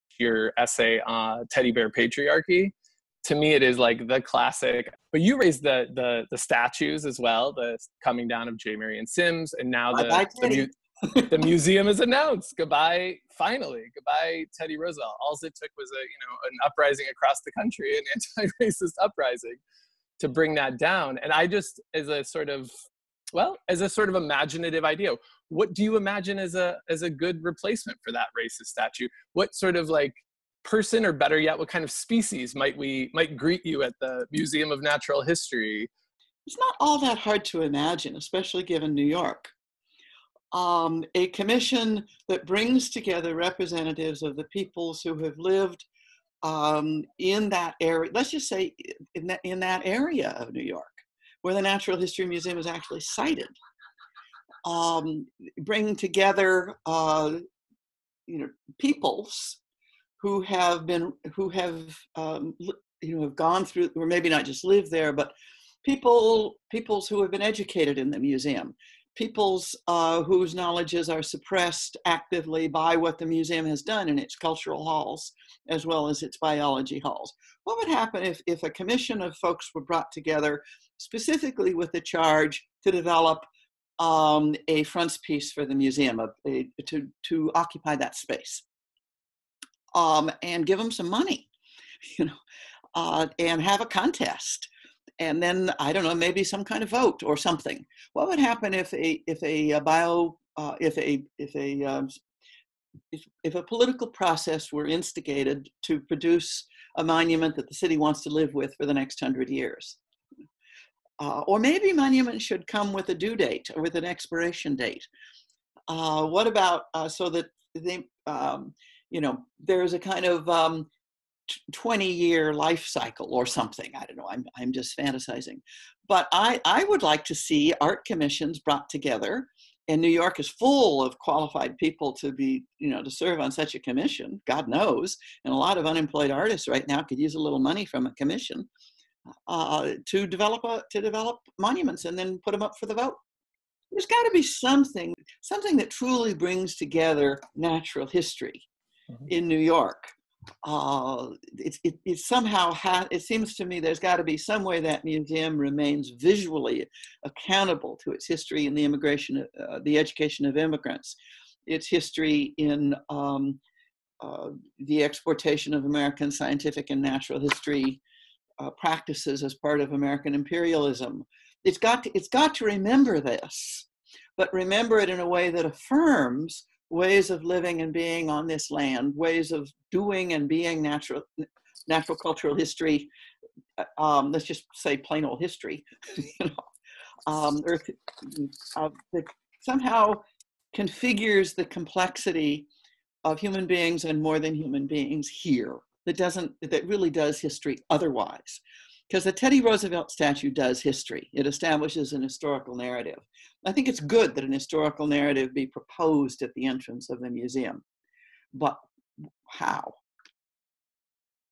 Teddy Bear Patriarchy: A Conversation With Donna Haraway
As anti-racist rebellions across the United States topple symbols of white supremacy, the statue of Theodore Roosevelt atop his horse remains at the entrance to the American Museum of Natural History. After countless protests and a long campaign, the museum has promised to remove it. And yet, as the museum reopens after a prolonged closure due to the COVID-19 crisis, it has yet to announce a date when the statue will be removed, a plan for where it will end up, and what might replace it.

Science for the People’s acquisitions editor, Erik Wallenberg, spoke with Donna Haraway, Professor Emerita in the History of Consciousness program at UC Santa Cruz and author of the article "Teddy Bear Patriarchy: Taxidermy in the Garden of Eden, New York City, 1908-36.” Published in 1984, the essay helped fuel a discussion about racism, colonialism, and white supremacy in natural history museums and in science more generally.

Here, we revisit that article and Haraway considers the role of art and artists in relation to science and the museum, and discusses how and with what Roosevelt might be replaced.